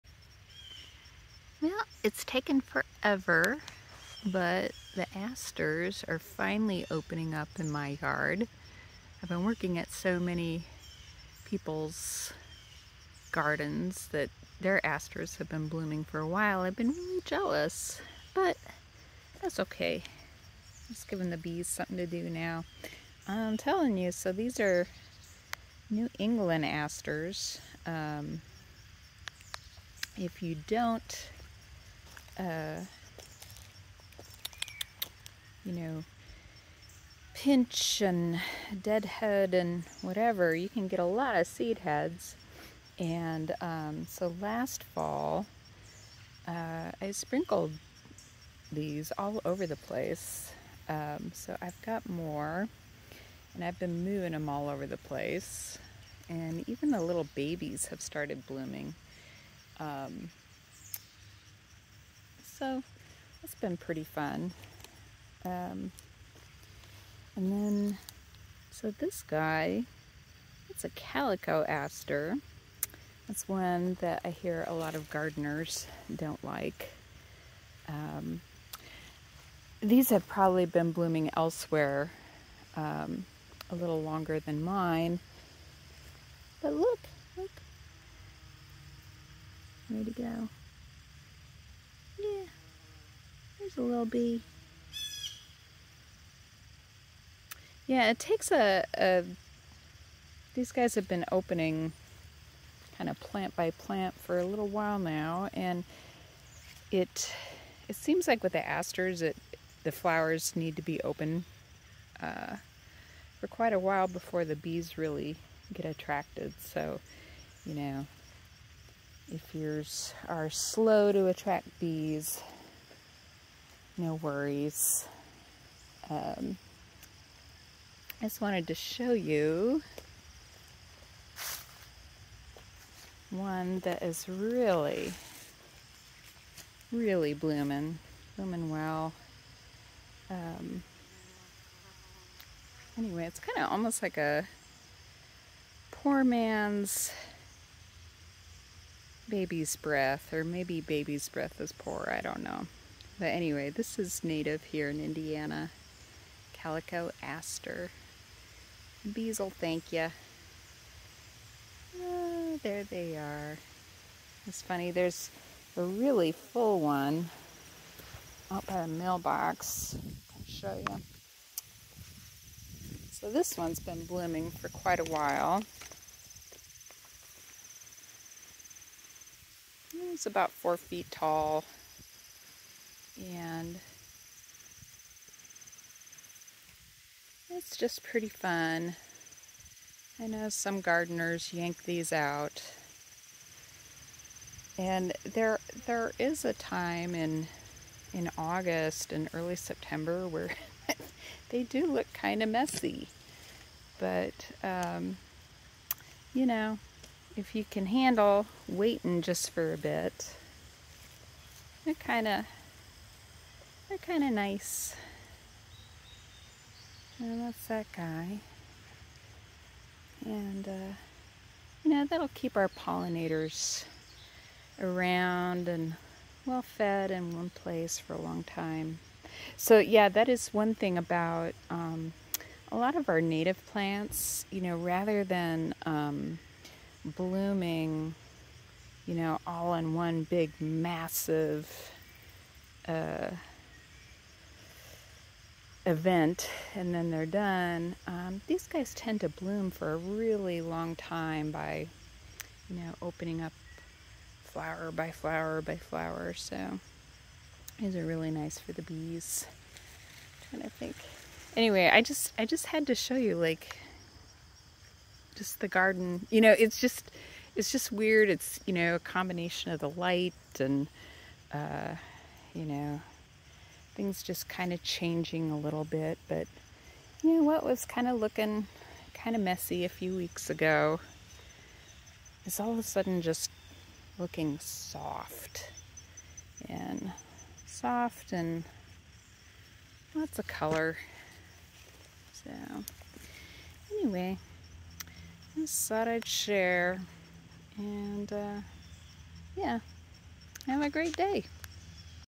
Fall Asters
Many people are familiar with New England asters, but quite a few folks will dig out calico asters. I have both blooming right now, and our pollinators love them both.